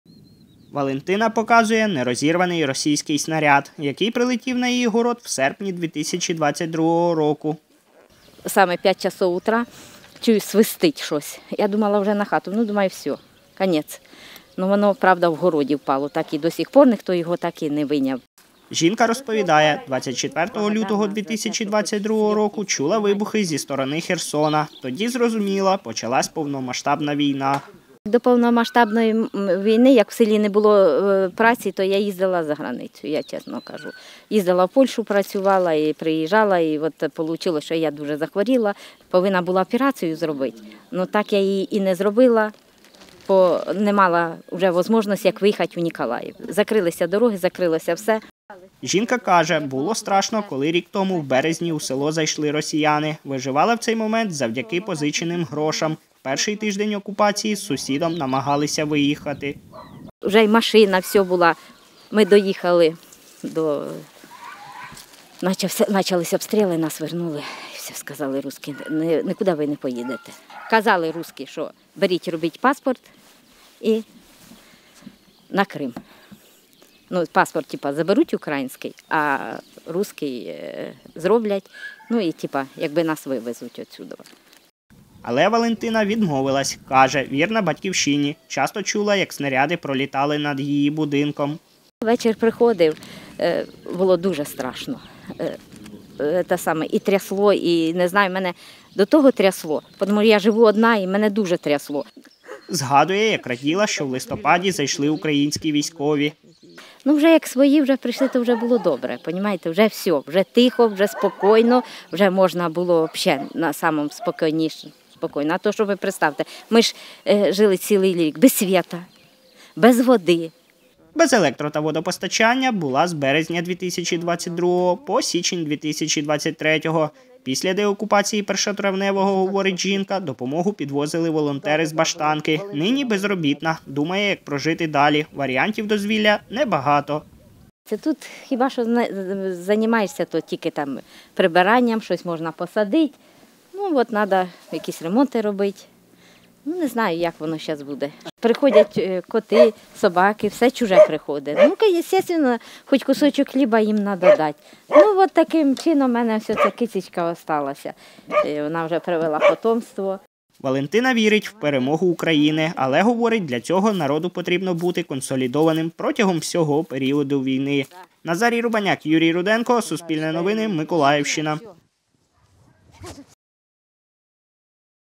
На Миколаївщині жінці довелось пережити окупацію
Село Першотравневе, що на Миколаївщині, російські війська окупували 15 березня 2022 року. Виїхати з села російські солдати дозволяли лише через Крим. Тому довелось виживати без світла, води й під обстрілами.